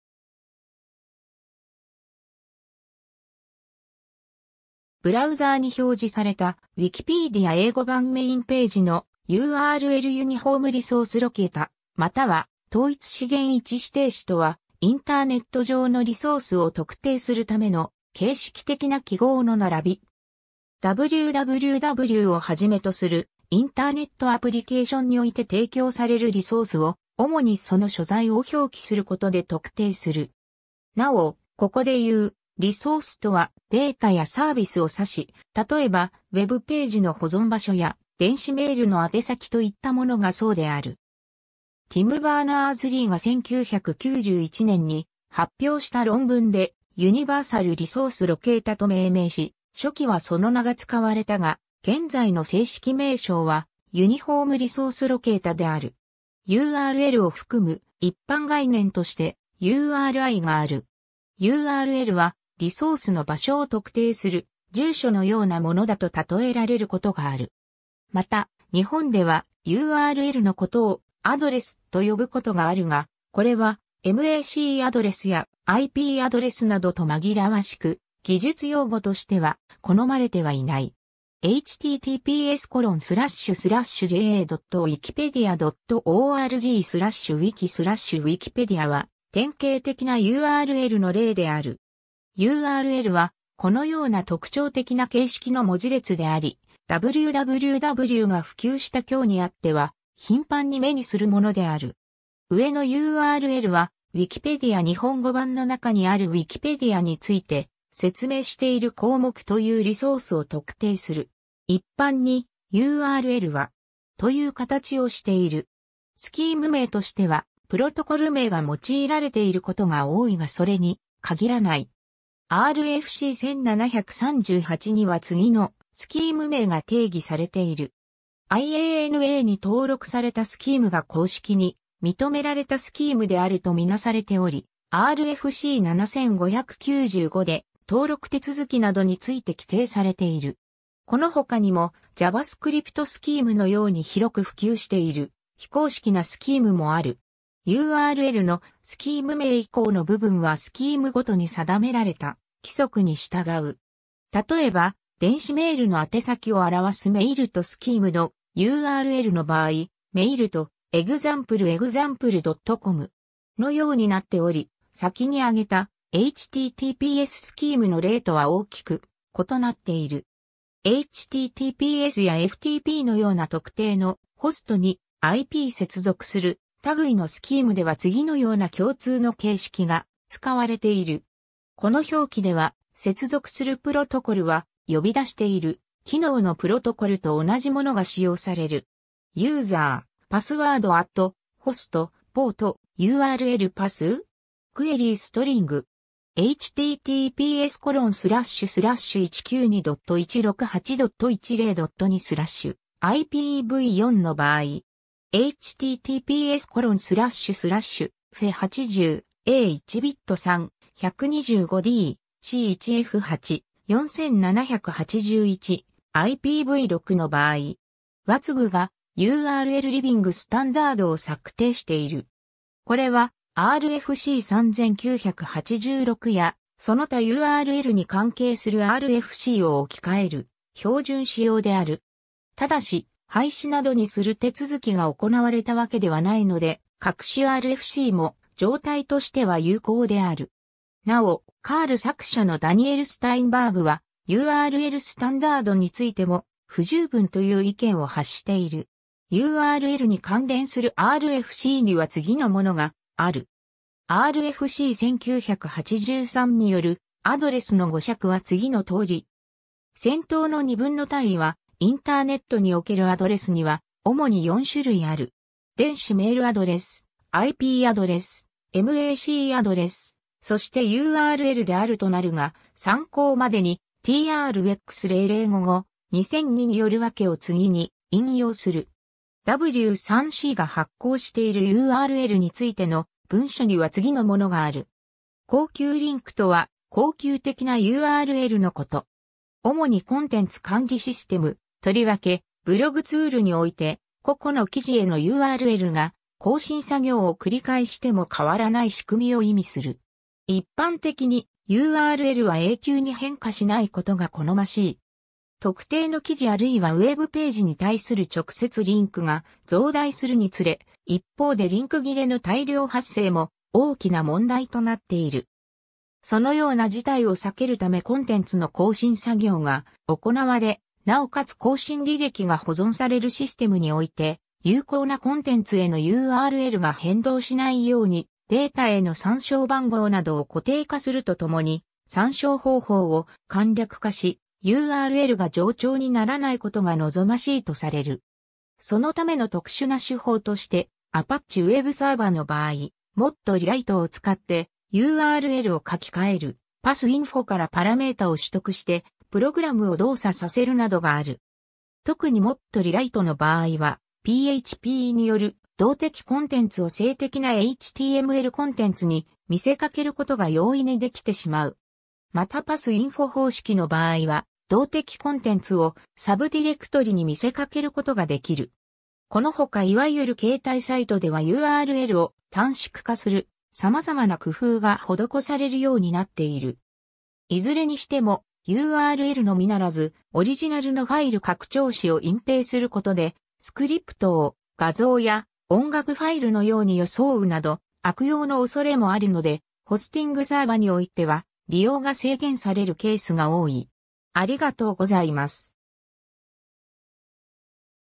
Uniform Resource Locator
ティム・バーナーズ＝リー
ブラウザーに表示された、WIKIPEDIA 英語版 メインページのURL Uniform Resource Locator（ユニフォーム リソース ロケータ、URL）または、統一資源位置指定子（とういつしげんいちしていし）とは、インターネット上のリソース（資源）を特定するための形式的な記号の並び。
WWWをはじめとするインターネットアプリケーションにおいて提供されるリソースを、主にその所在を表記することで特定する。
なお、ここでいう、「リソース」とは、（主にインターネット上の）データやサービスを指し、例えばウェブページの保存場所や電子メールの宛先といったものがそうである。
 ティム・バーナーズ＝リーが1991年に発表した論文でUniversal Resource Locatorと命名し、初期はその名が使われたが、現在の正式名称は、「Uniform Resource Locator」である。
 URLを含む一般概念としてURIがある。
 URLはリソースの場所を特定する「住所」のようなものだと例えられることがある。
 また、日本ではURLのことを「アドレス」と呼ぶことがあるが、これは、MACアドレスやIPアドレスなどと紛らわしく、技術用語としては、好まれてはいない。
URLはこのような特徴的な形式の文字列であり、WWWが普及した今日にあっては頻繁に目にするものである。
 上のURLは「ウィキペディア日本語版の中にあるウィキペディアについて説明している項目」というリソースを特定する。
 一般にURLは （スキーム名）:（スキームごとに定められた何かの表現形式）
 という形をしている。
スキーム名としてはプロトコル名が用いられていることが多いがそれに限らない。
 RFC 1738には次のスキーム名が定義されている。
 IANAに登録されたスキームが公式に認められたスキームであると見なされており、 RFC 7595で登録手続きなどについて規定されている。
この他にもjavascriptスキーム（この後ろに書かれた内容がJavaScript言語によって書かれたスクリプトであることを示す）のように広く普及している非公式なスキームもある。
 URLの、スキーム名以降の部分はスキームごとに定められた規則に従う。
 のようになっており、先に挙げたhttpsスキームの例とは大きく異なっている。
 httpsやftpのような特定のホストにIP接続する類のスキームでは次のような共通の形式が使われている。
この表記では、接続するプロトコルは、呼び出している機能のプロトコルと同じものが使用される。
 //「user」:「password」@「host」:「port」/「url-path」?「query-string」
 WHATWGがURL Living Standardを策定している。
これは、RFC 3986やその他URLに関係するRFCを置き換える標準仕様である。
ただし、廃止などにする手続きが行われたわけではないので、各種RFCも状態としては有効である。
 なお、cURL作者のDaniel SteinbergはURL Standardについても不十分という意見を発している。
 URLに関連するRFC（およびその邦訳）には次のものがある。
 RFC 1983による"address"の語釈は次の通り（プレーンテキストの原文に太字の効果を付与し、1行文字数などの体裁を調整）。
 先頭の2文の大意は、「インターネットにおけるアドレスには主に4種類ある。電子メールアドレス、IPアドレス、MACアドレス、そしてURLである」となるが、参考までに、TR X 0055:2002による訳を次に引用する（太字は引用者）。
 W3Cが発行しているURLについての文書には次のものがある。
 恒久リンク（英: permanent link）とは恒久的なURLのこと。
主にコンテンツ管理シス...